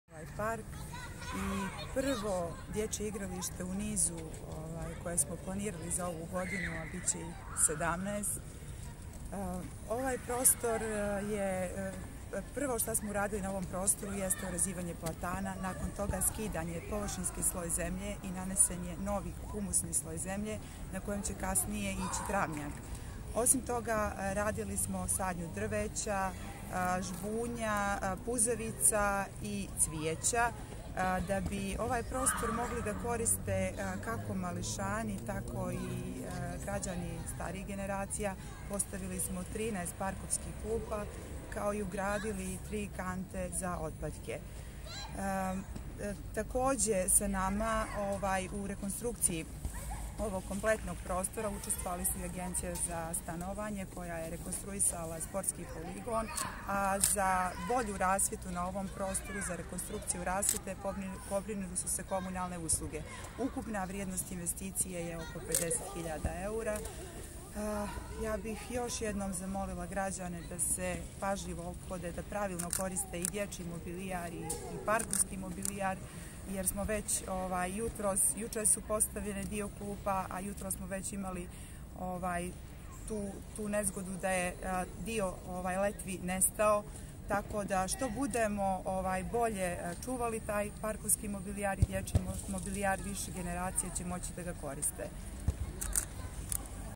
Daca Popović
Izjava direktorice gradskog preduzeća Zelenilo doo povodom otvaranja novog parka sa dječjem igralištem i poligonom u Bloku N

Podgorica, subota, 29. februar 2020. godine